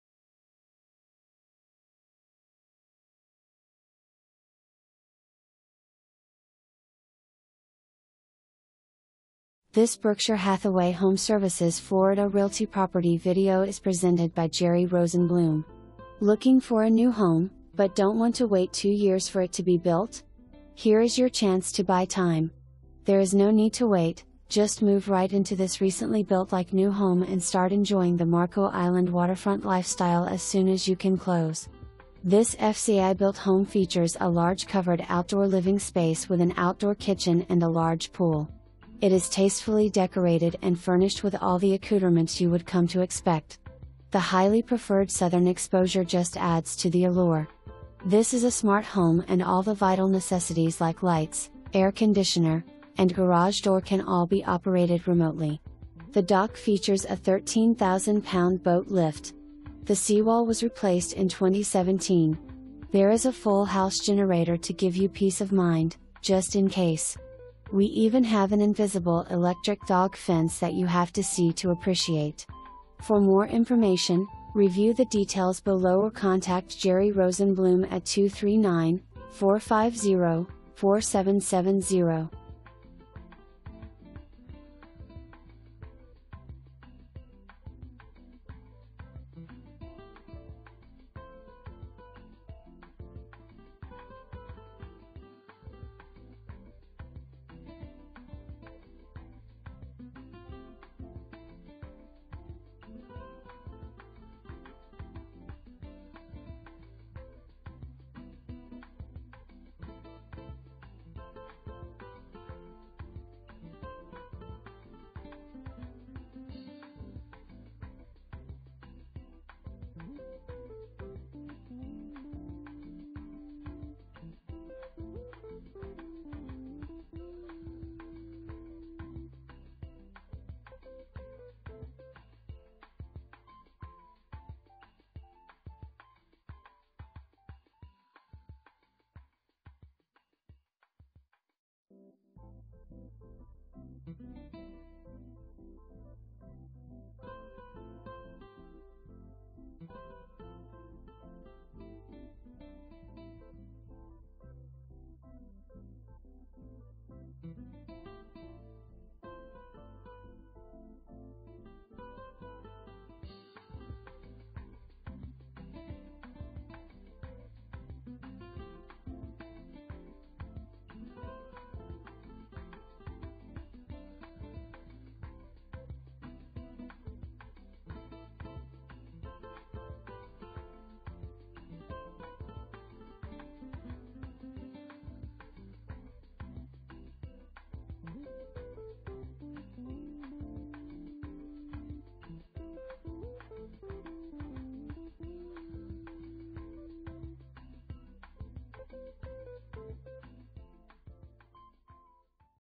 Homes for Sale - 1641 Orleans CT, MARCO ISLAND, FL
Looking for a new home, but don't want to wait 2 years for it to be built? Here is your chance to buy time. There is no need to wait, just move right into this recently built like-new home and start enjoying the Marco Island waterfront lifestyle as soon as you can close. This FCI-built home features a large covered outdoor living space with an outdoor kitchen and a large pool. It is tastefully decorated and furnished with all the accouterments you would come to expect. The highly preferred Southern exposure just adds to the allure. This is a ''smart'' home and all the vital necessities like lights, AC, and garage door can all be operated remotely. The dock features a 13,000-pound boat lift. The seawall was replaced in 2017. There is a full-house generator to give you peace of mind, just in case... We even have an invisible electric dog fence that you have to see to appreciate.

3 full bath

Gerry Rosenblum
Berkshire Hathaway HomeServices Florida Realty